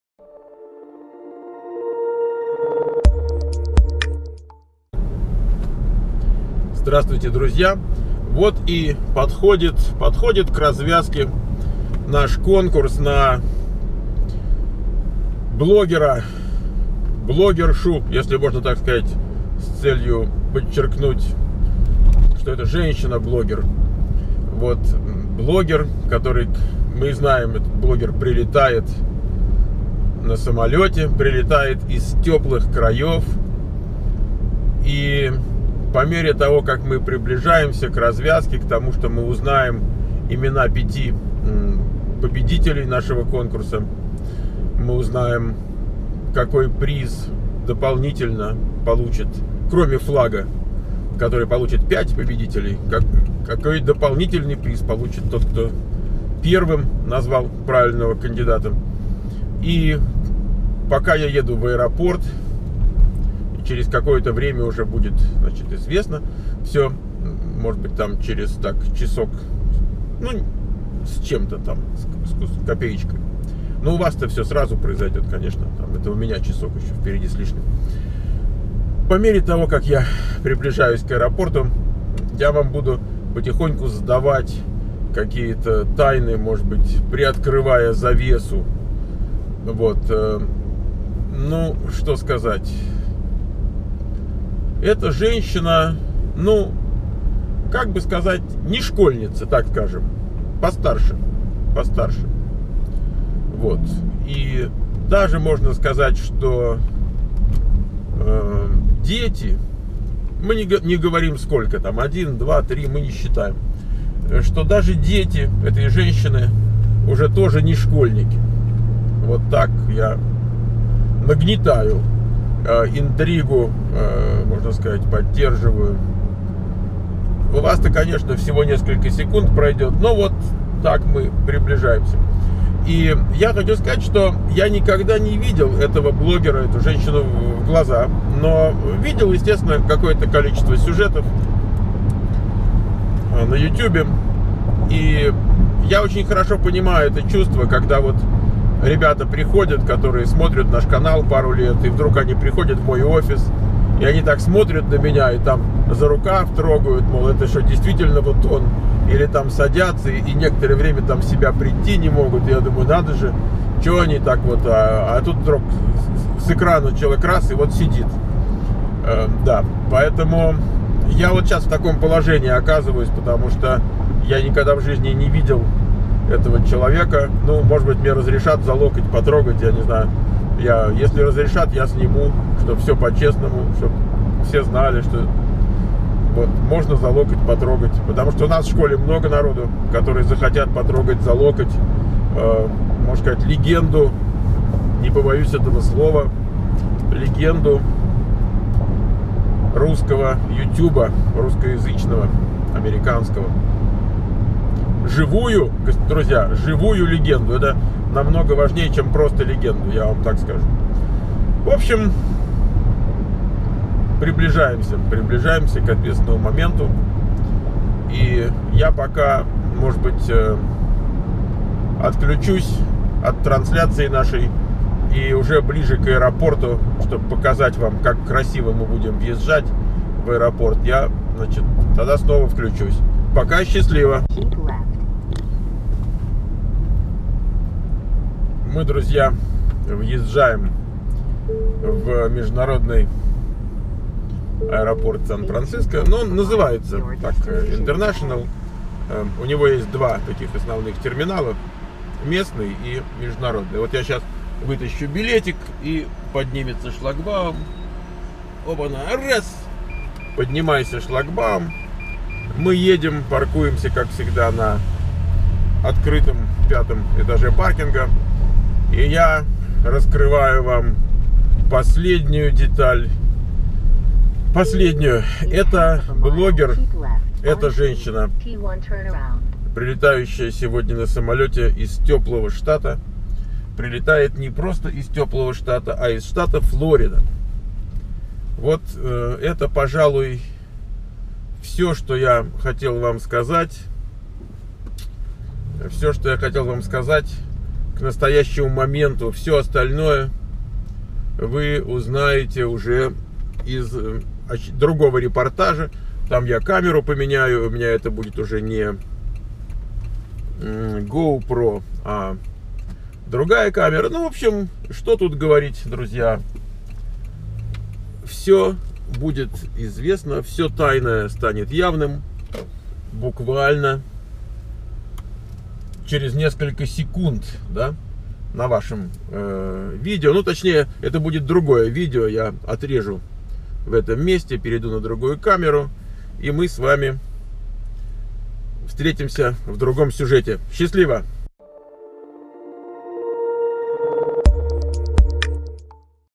США 2416: Мы узнаем больше подробностей о прибывающем в Кремниевую Долину блоггере
Мы узнаем больше подробностей о прибывающем в Кремниевую Долину блоггере